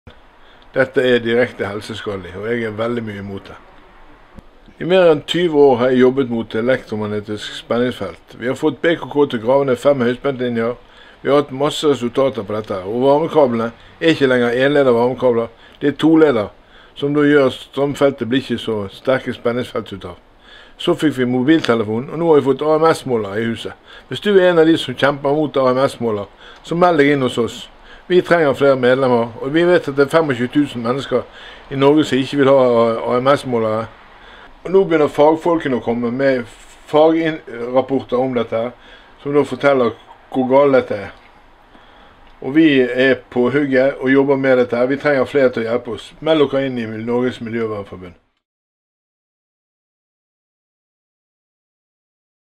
Video Kurt Oddekalv Stopp smartmålerne, bli medlem i NMF!
Vi arbeider for å senke elektromagnetisk stråling. Hjelp oss i Norges Miljøvernforbund til å bli enda bedre på dette arbeidet. Bli medlem!